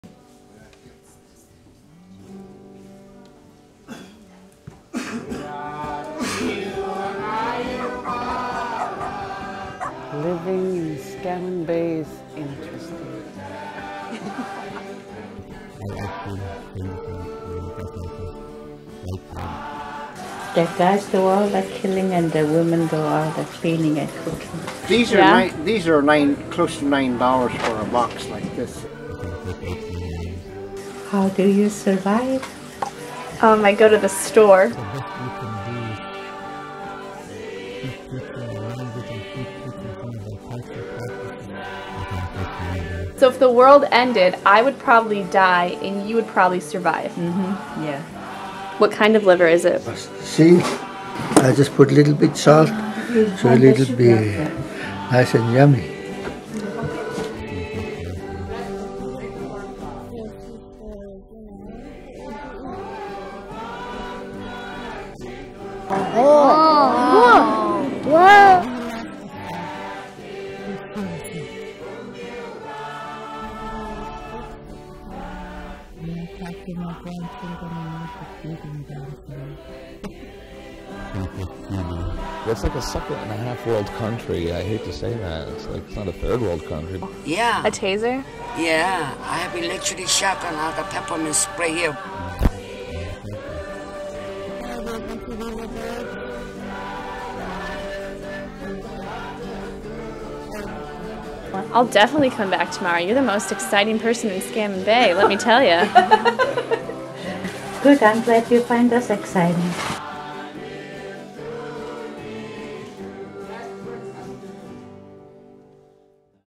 Scammon Bay Trailer
This is the trailer for my senior project. I went to Scammon Bay, Alaska and shot a documentary.